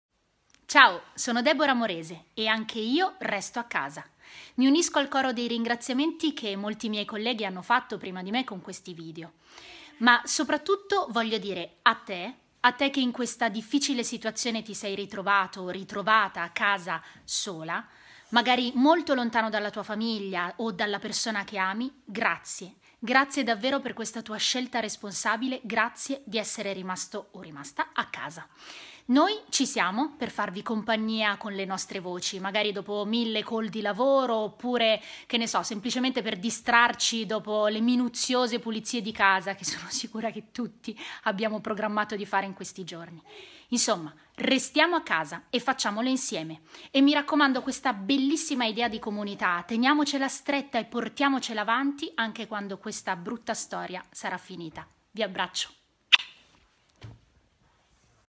CORONAVIRUS: l'appello di DEBORAH MORESE
IORESTOACASA, è importante, parla la voce italiana di tanti eroi animati COVID19 - CONDIVIDI tra i tuoi contatti